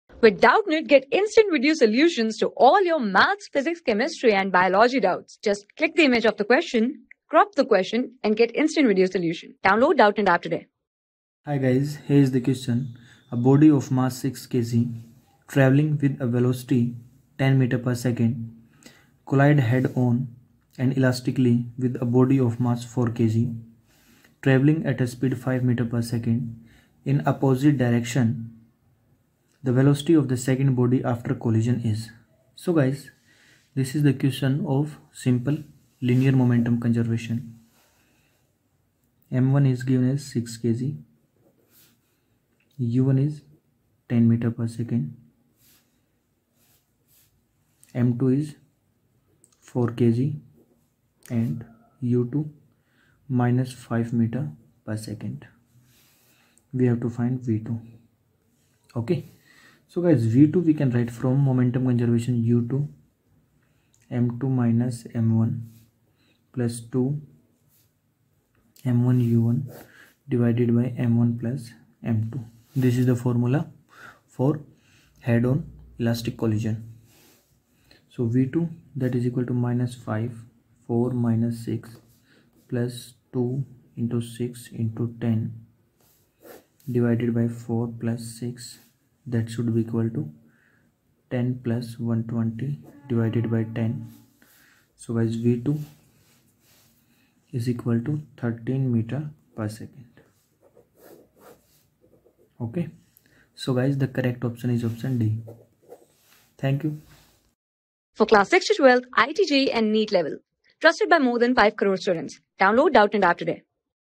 A body of mass 6kg travelling with a velocity 10 m/scollides head - on and elastically with a bo...
A body of mass 6kg travelling with a velocity 10 m/scollides head - on and elastically with a body of mass 4kg travelling at a speed 5 m/s in opposite direction. The velocity ofthe second body after the collision is
Class: 11
Subject: PHYSICS
Chapter: WORK, POWER & ENERGY
Board:IIT JEE

👉 Have Any Query? Ask Us.